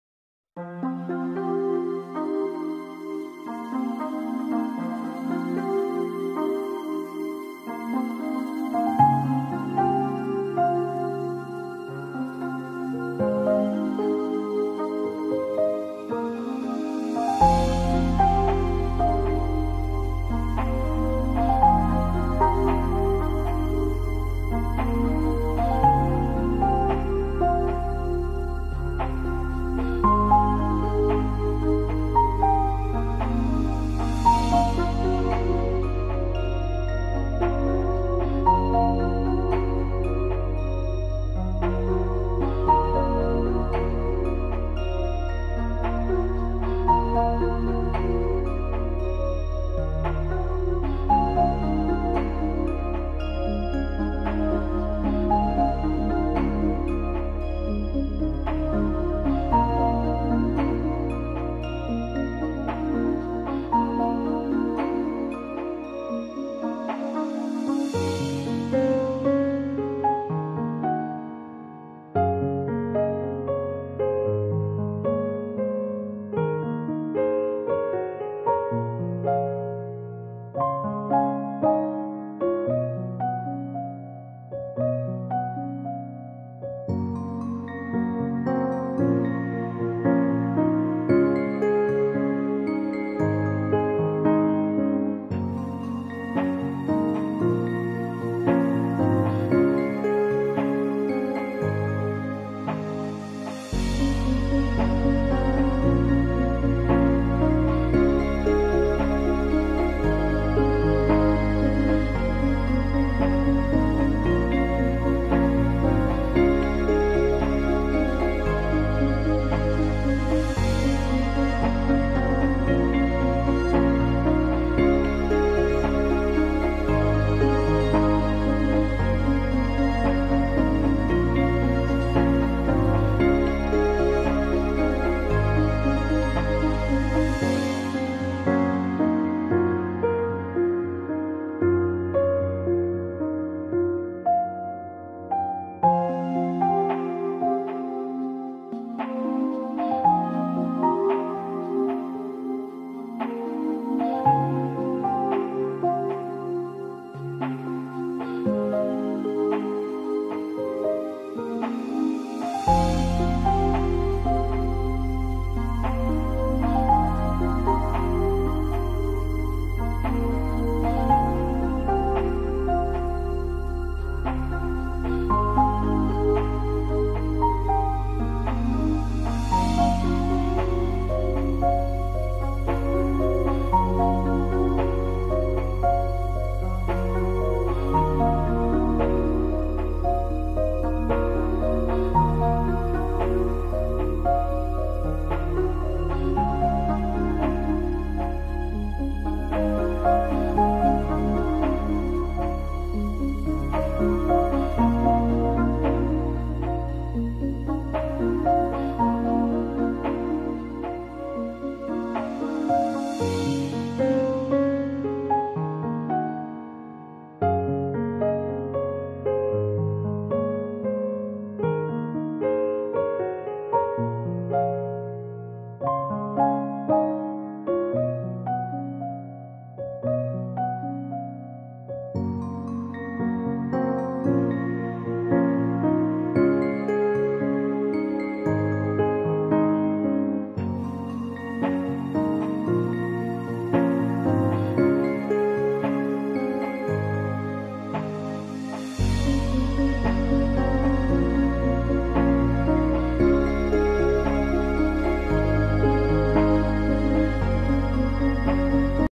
All Times FIFA World Cup Tropies
All Times FIFA World Cup Tropies from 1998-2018, Brazil 5, Germany 4, Italy 4, ...